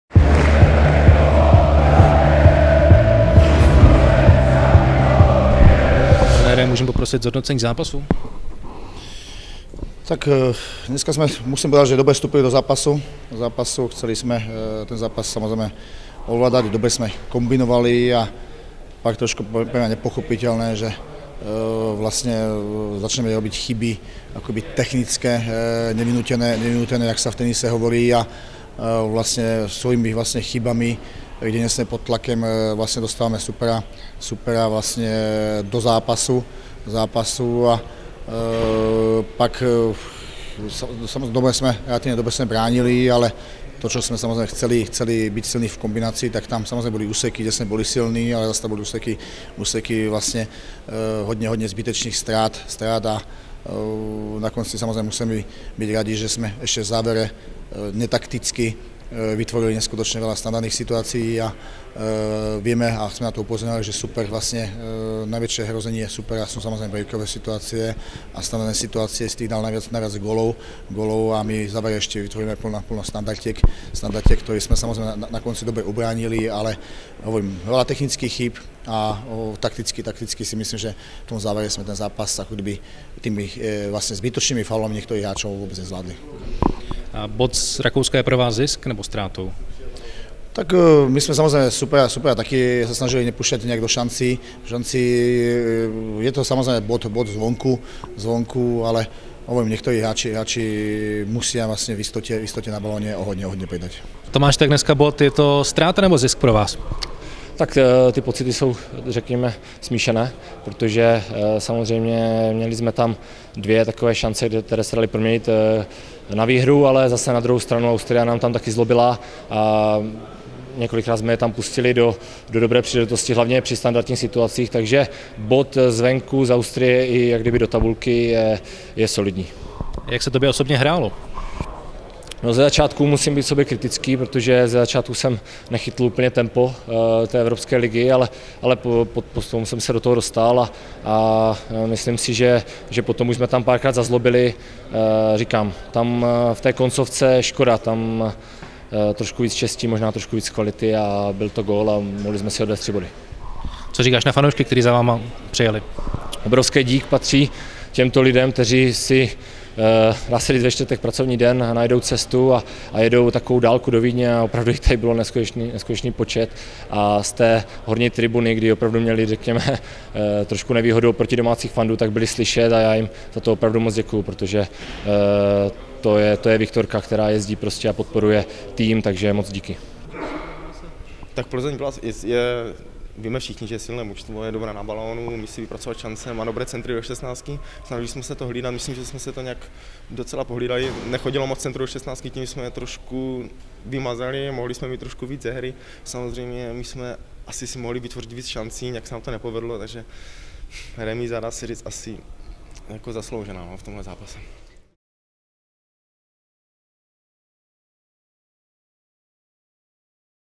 Ohlasy po remíze 0:0 Viktorie ve Vídni
Roman Pivarník, Tomáš Poznar a Patrizio Stronati hodnotí bezbrankovou remízu utkání Evropské ligy.